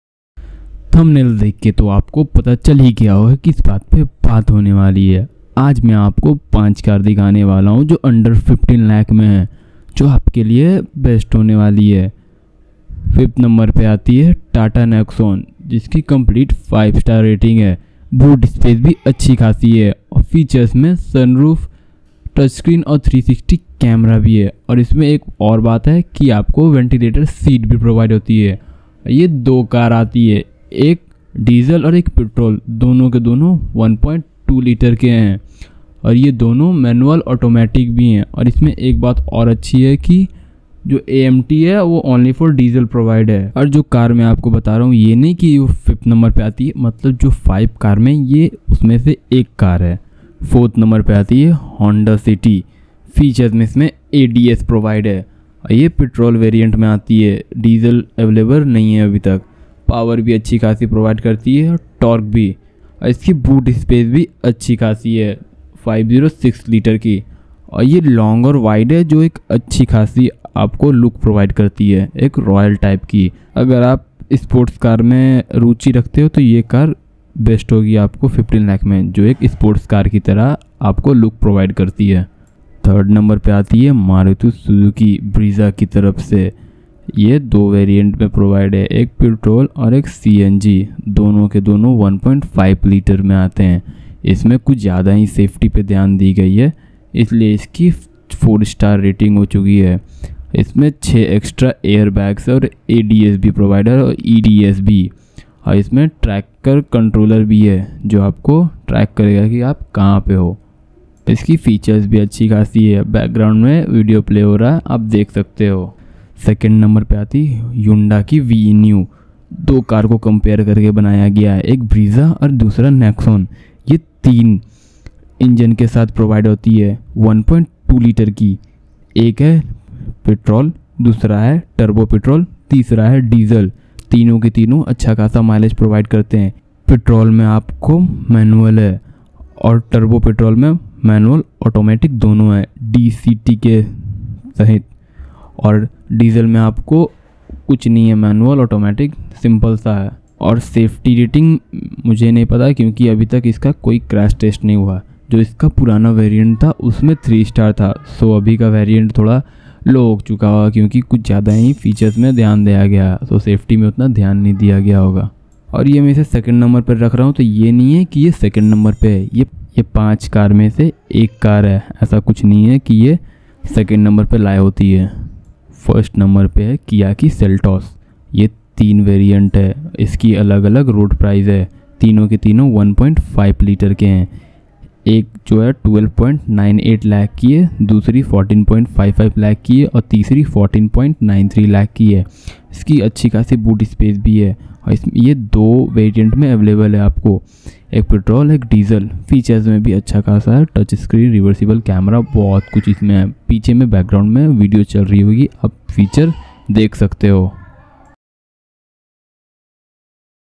In this video, we share with you the Top 5 cars in Rs.15 lakh budget in the market right now!
topp five car under 15 lakh
taat nexon
honda city
hyunda venue
kia selto